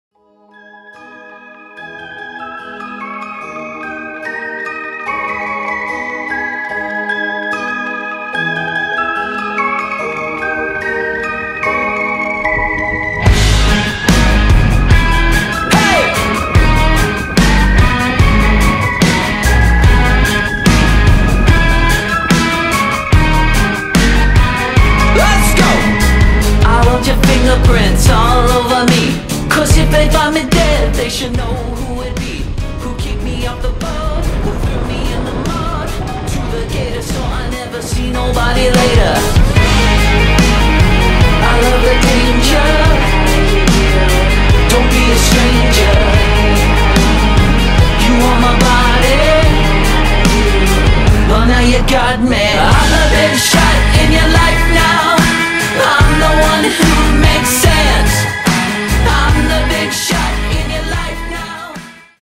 EPIC CLOSEOUT BARRELS! (Trigg Beach)
Epic offshore winds caused the waves to barrel up.